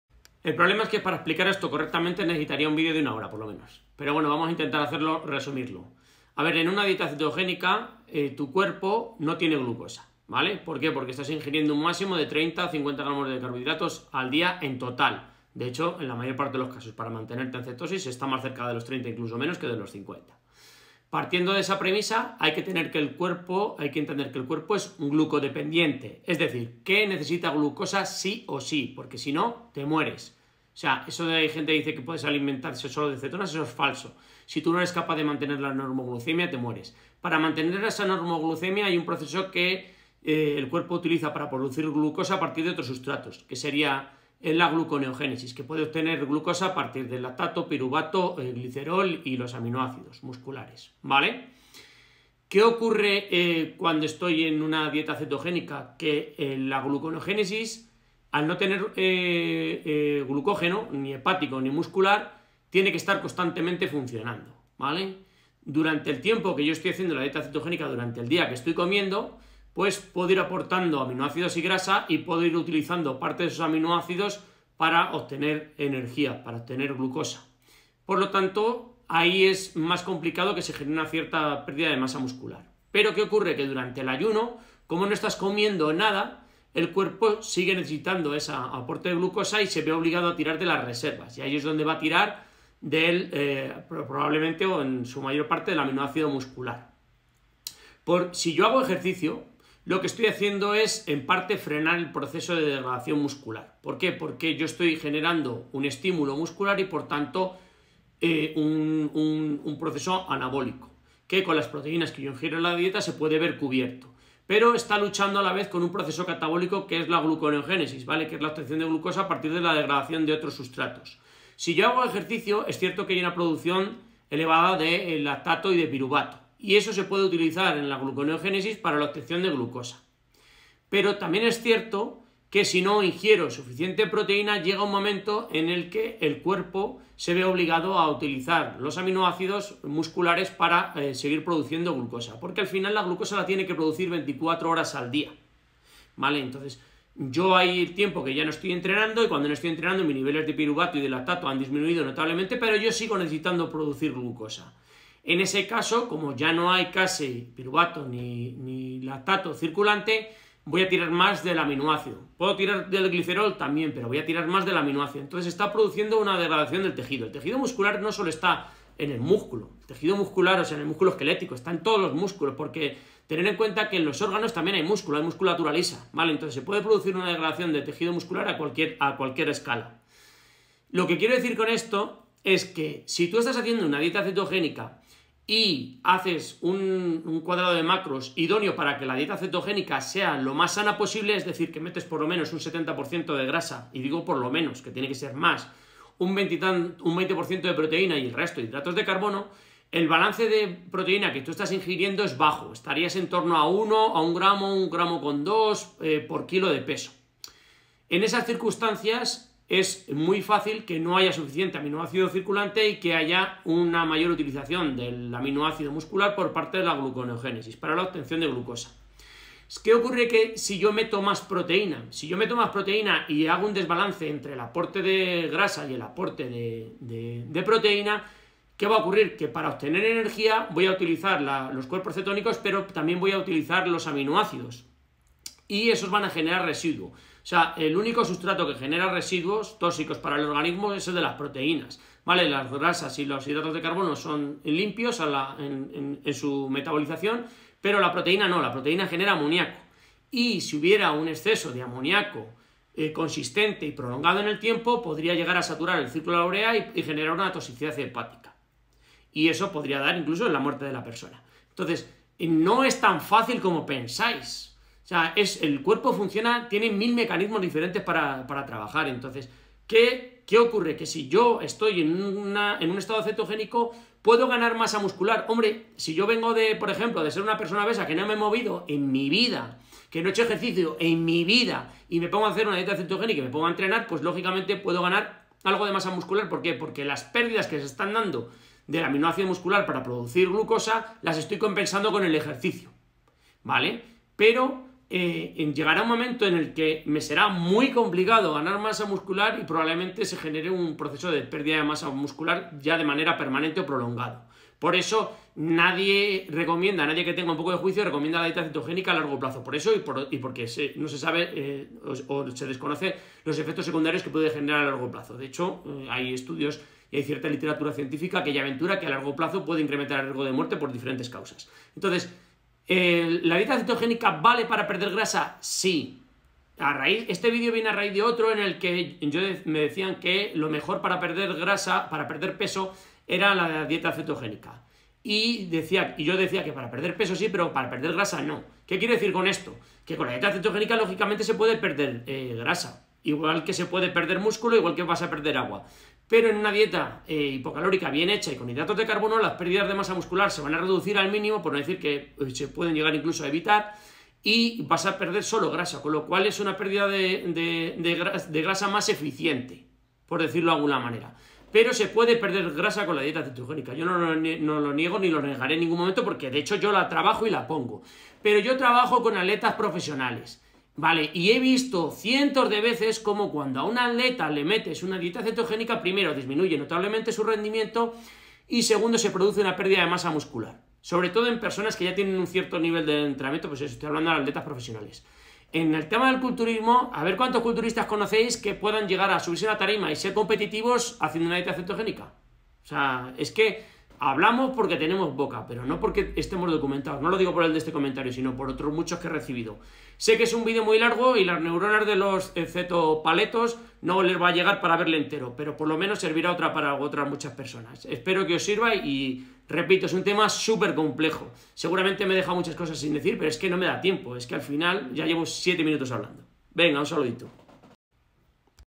Dieta keto y la pérdida de músculo
Explicación de porqué se pierde masa en la dieta keto